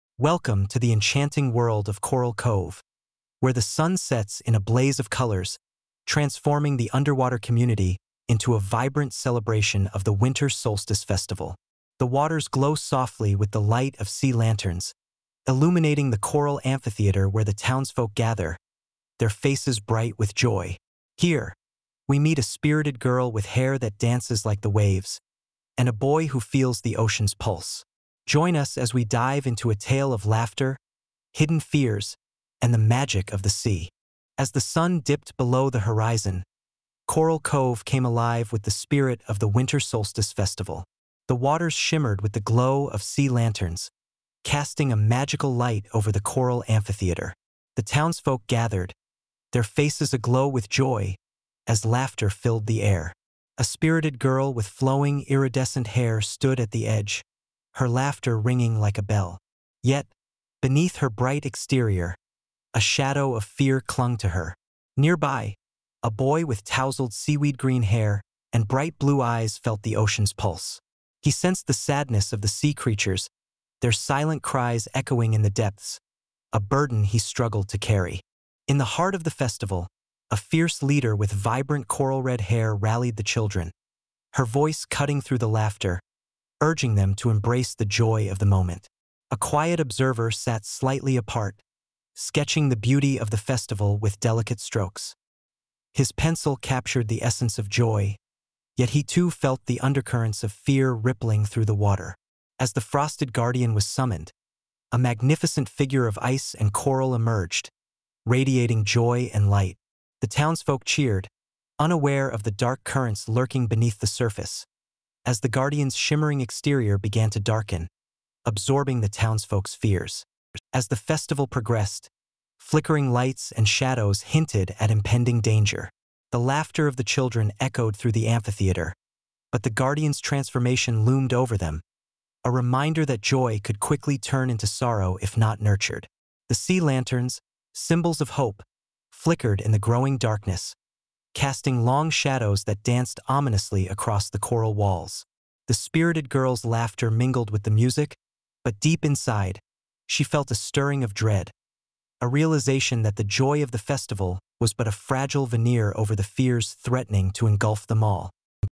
Joy and Hidden Fears of Coral Cove | Chapter 1 - Winter Solstice Celebration (2024)
Join the enchanting Winter Solstice celebration in Coral Cove, where joy masks hidden fears beneath the shimmering waters. Dive into a tale of courage, unity, and the magic of friendship!

In this captivating first chapter, we meet spirited children in Coral Cove as they celebrate the Winter Solstice festival, unaware of the shadow of fear lurking beneath the surface.

Key Points:
- A vibrant festival setting filled with joy
- Introduction of characters Luna, Finn, Coral, and Nemo
- Discovery of unshared fears and hidden emotions
- Signal of impending danger through the Guardian's transformation

Timestamps:
00:00 - Intro & Hook
00:45 - The Festival Begins
01:30 - Laughter and Hidden Fears
02:15 - Guardian's Transformation